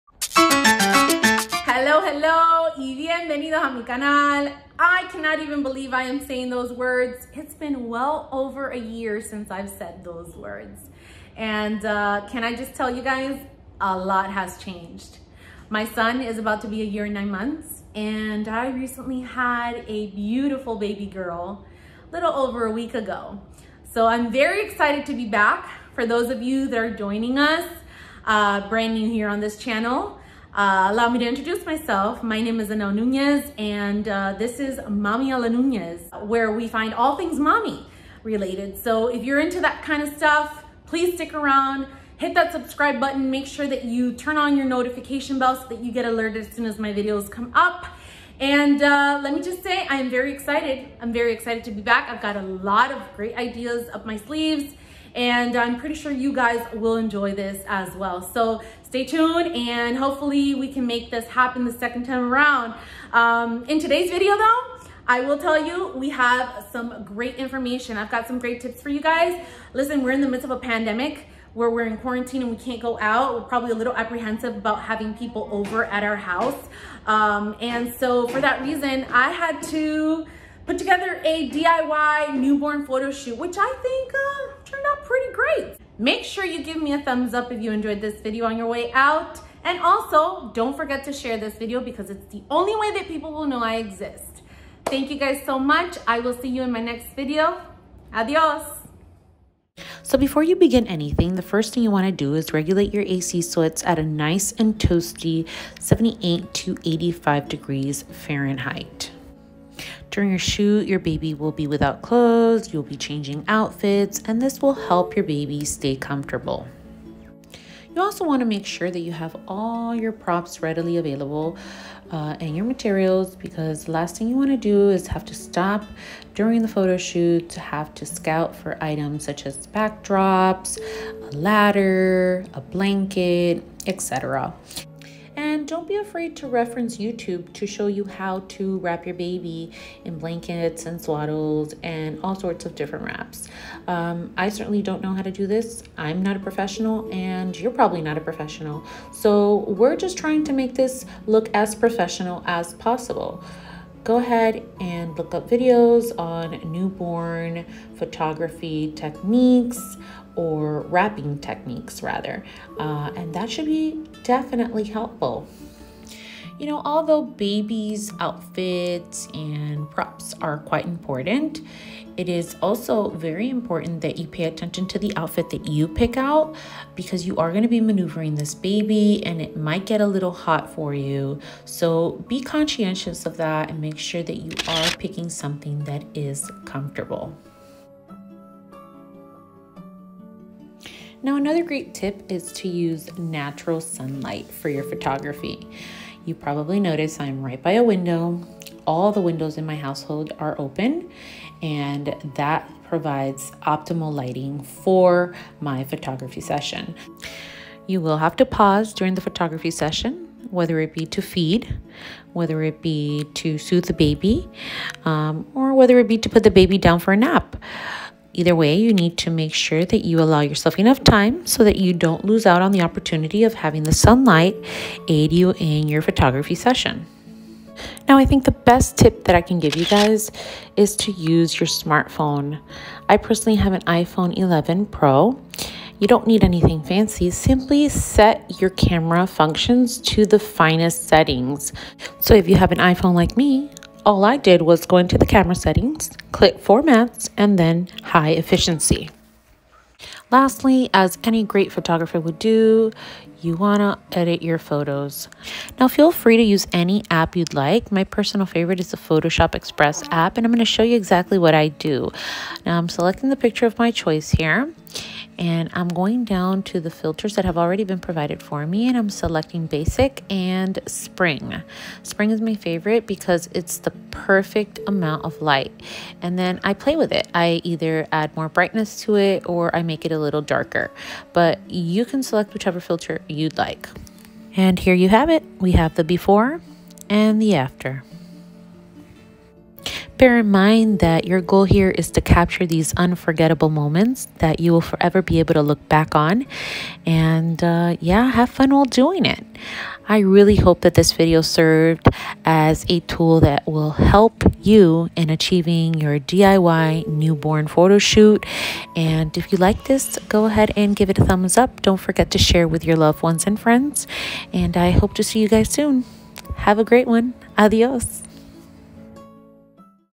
DIY | Newborn Photoshoot
Hola (hello)!

Guess who's back?! It's been a while and I'm coming back after a year hiatus with some news and a DIY newborn photoshoot session (including great tips). I hope you find this video helpful. :)